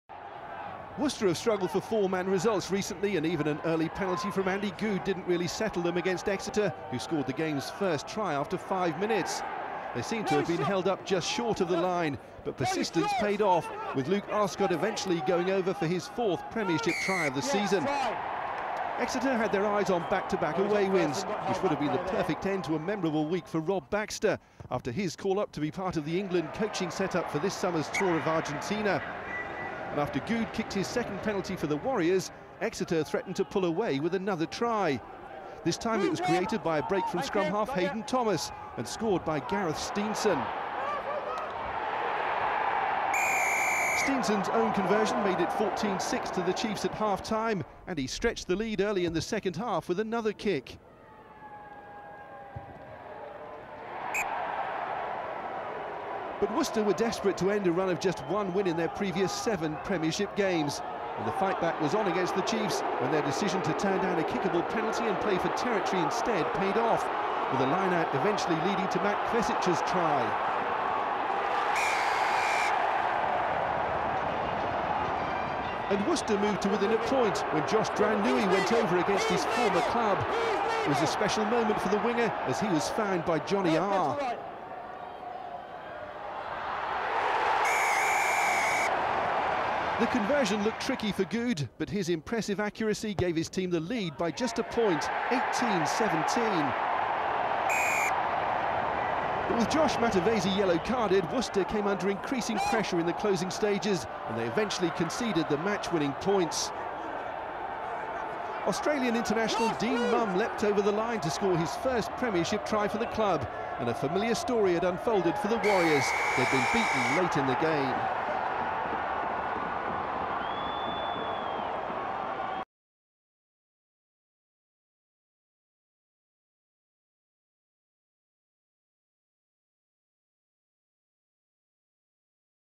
Worcester Warriors vs Exeter Chiefs 18 - 24 | Premiership Rugby Round 19
Dean Mumm's late try proved the difference as the Exeter Chiefs edged a topsy-turvy encounter at Sixways to emerge 24-18 victors against the Worcester Warriors.

The Australian international barrelled over with only three minutes remaining to put Rob Baxter's side back in front after it looked like they had thrown their lead away.
First-half tries from Luke Arscott and Gareth Steenson had put the Chiefs on course for their first back-to-back away victories in the Aviva Premiership since last April.

But the boot of Andy Goode and tries from Matt Kvesic and Josh Drauniniu brought the hosts right back into it as they moved 18-17 ahead on the hour mark.

A sterling defensive effort in the final ten minutes with Josh Matavesi in the sin bin looked like it might deny the Chiefs but eventually the pressure told and Mumm went over for the win.

The game had started at breakneck speed, as early as the second minute Goode put the Warriors in the lead with a penalty from bang in front after the Chiefs infringed at the breakdown.

But the Chiefs response was immediate, a rolling maul from a lineout ending with Luke Arscott burrowing over right under the posts, Steenson making no mistake with the extras to make it 7-3.

Moments later David Lemi looked like he had crossed for the Warriors' first try of the match, after Goode's clever dink to the corner had unlocked the Chiefs' defence.

But the referee decided Lemi had knocked on in the process of grounding the ball and instead gave Exeter the feed at the scrum.

However Jack Nowell looked to run the ball from deep and was pinged for playing on the ground leaving Goode another simple penalty for 6-7.

The frantic start to the first half showed no sign of abating and it was the visitors who struck next on 17 minutes.

Scrum-half Haydn Thomas broke through a gap in the Warriors line before offloading to Steenson who went over under the posts, adding the conversion for 14-6.

Goode's long-range penalty drifted wide before half time as the Warriors fought to reduce the deficit but Steenson's penalty after the interval put the Chiefs well clear at 17-6.

But the Warriors much-needed response was immediate, Matavesi joining a Warriors rolling maul from a lineout and helping to push Kvesic over, Goode missing the conversion from the touchline.

The Warriors were on the up now and full-back Chris Pennell went streaking clear and it looked for a moment like he was about to put Lemi in for a try but the Chiefs' scramble defence came to their rescue.

But the Warriors pressure was continuous, Goode turning down a shot at goal to kick to the corner after his fine break had pushed them deep into Chiefs territory.

And after Baxter's side had quelled the initial thrust of the lineout Drauniniu snuck over in the corner, Goode's tricky conversion putting the hosts back in front at 18-17 on the hour.

In the closing stages the Chiefs pushed and probed in search of the decisive score and finally found it in the 77th minute as Mumm blasted over from close range after a series of pick and go drives from the Chiefs forwards.